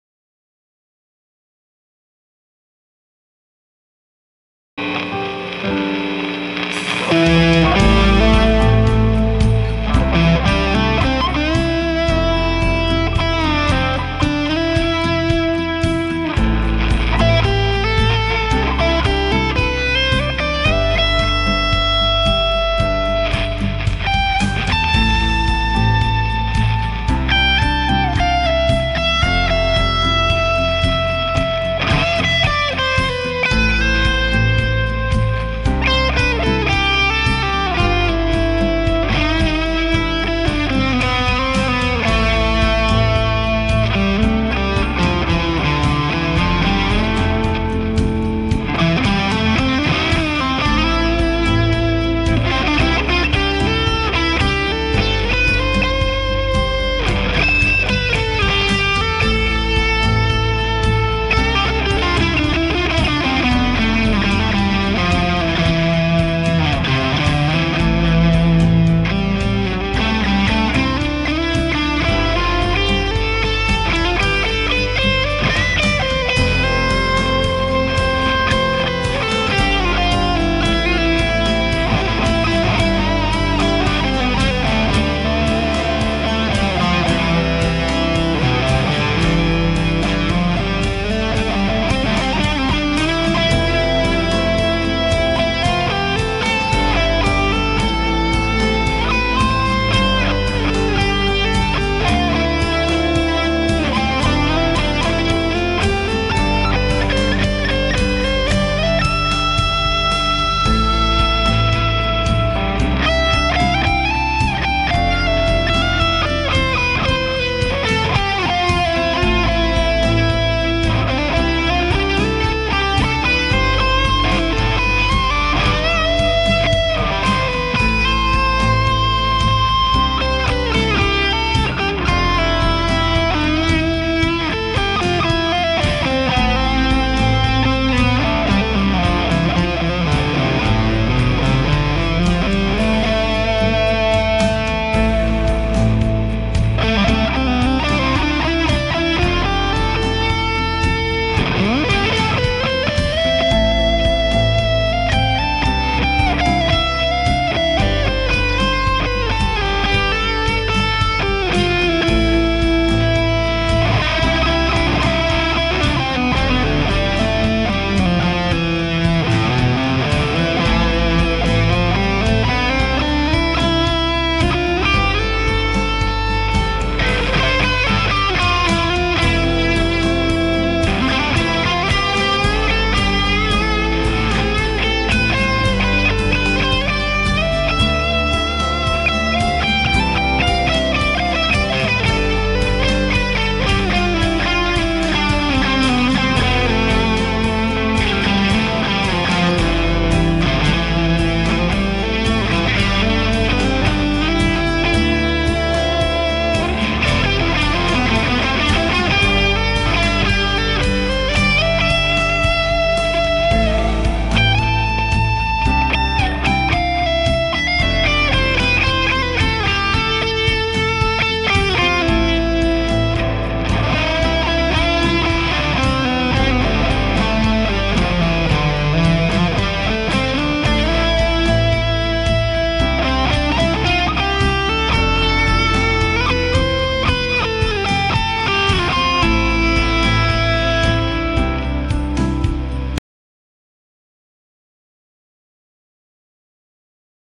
〖トレブルブースター+FUZZ〗Fender Japan ST62 55 1989（改）＆Kz TrebleBooster+Flyingteapot FUZZ 即興アドリブギター001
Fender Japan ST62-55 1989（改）
Kz GuiterWorks Kz TrebleBooster
Flyingteapot FUZZ
Boss GE-10
Maxon DD-1000
Orenge DarkTerror
Marshall JCM9001960A